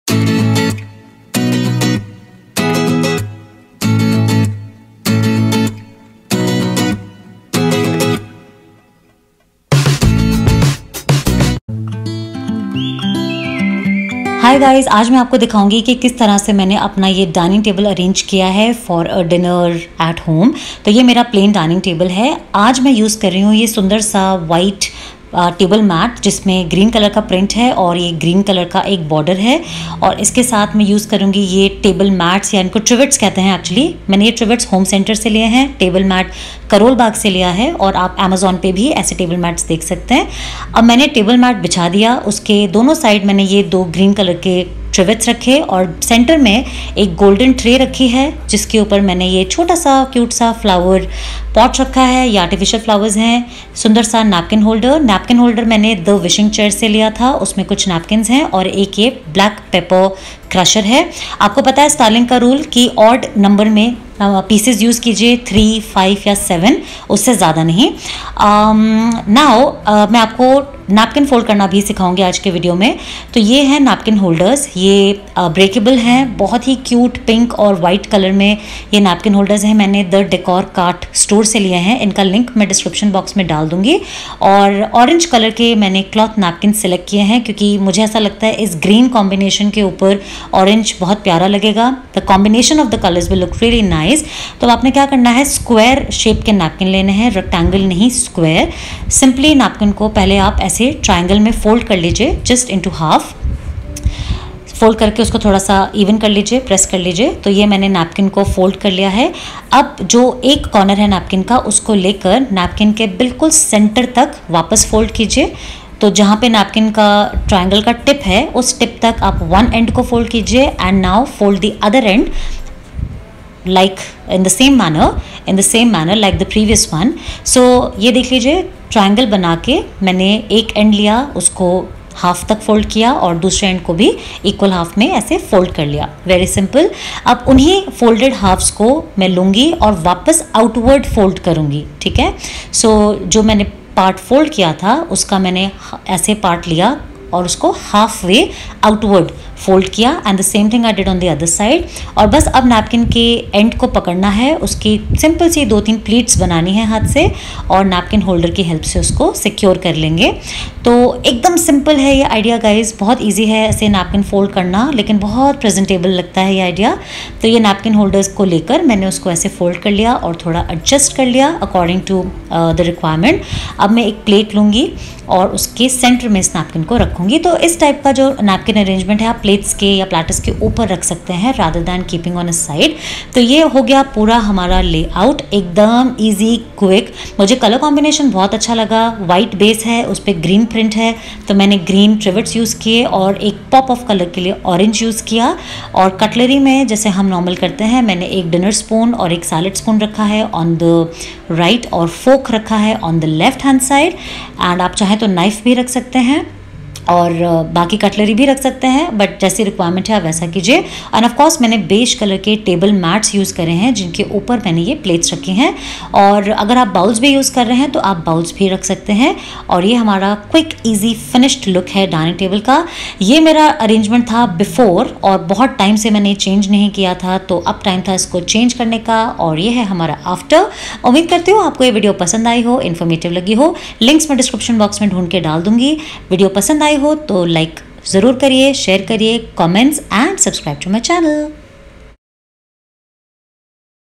DINING TABLE STYLING TIPS FOR GUESTS & NAPKIN FOLDING IDEA
Here is another home decor related video.I have shared a new way of styling/decorating dining table along with napkin folding ideas.

table covers: